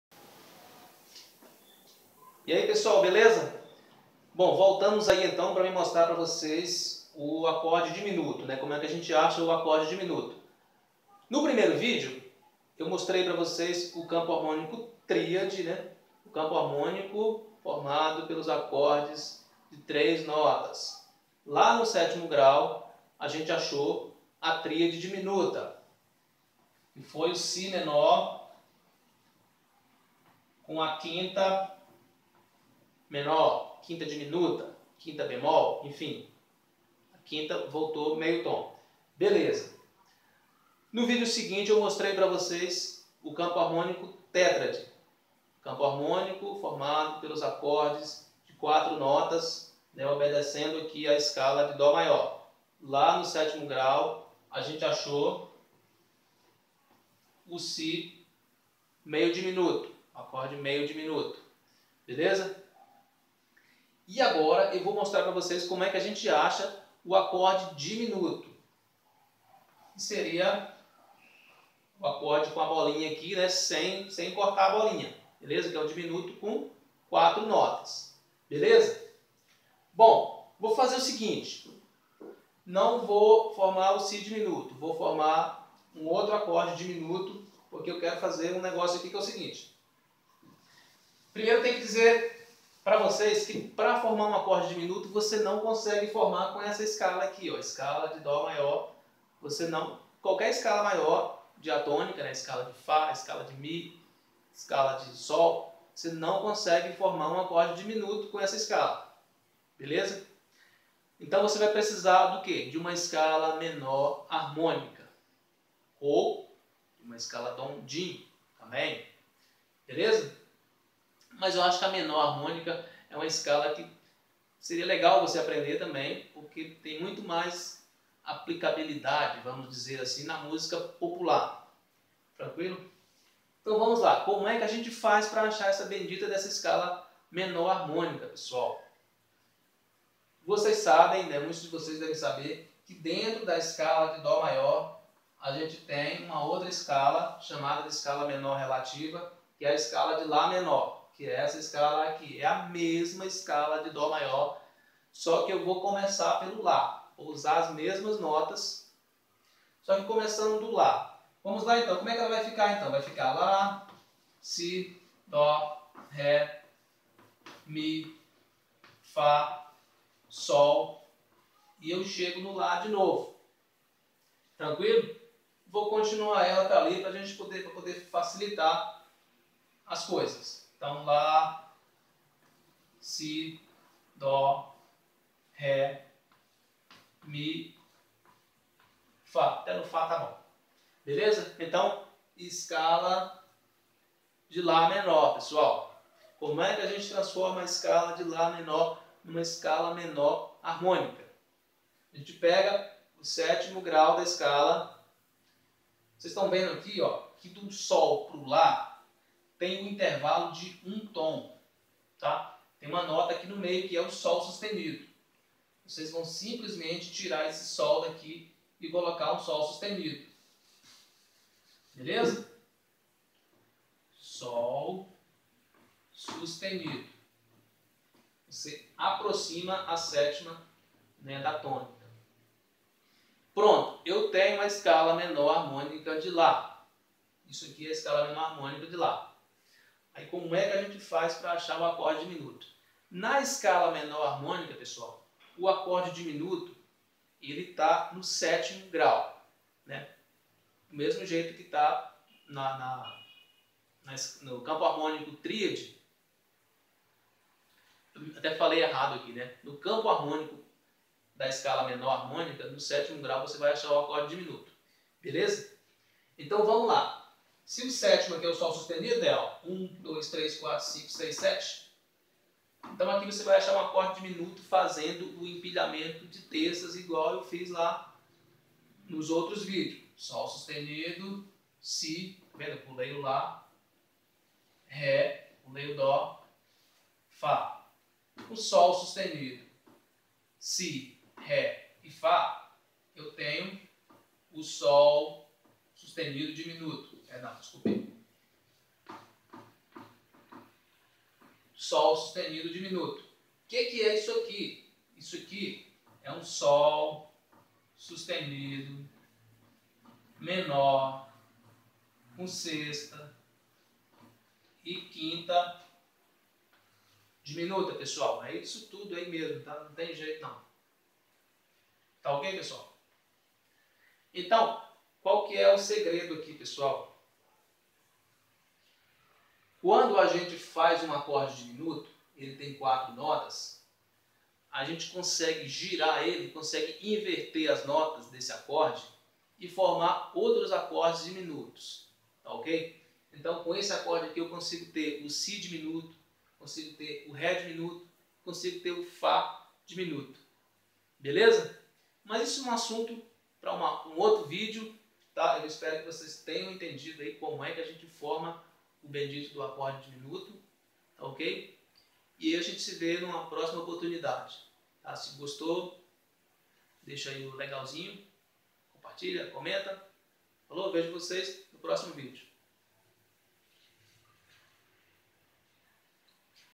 Como montar um acorde diminuto
Esse é o terceiro vídeo da série.  Nele explico como montar um acorde diminuto fazendo o empilhamento de terças com notas da escala menor harmônica.
Boas idéias pra vocês.

E-book Como Tocar Guarânia no violão

Guarânia - 2

E-book Guarânia para iniciantes de violão

Canal do Campo Harmônico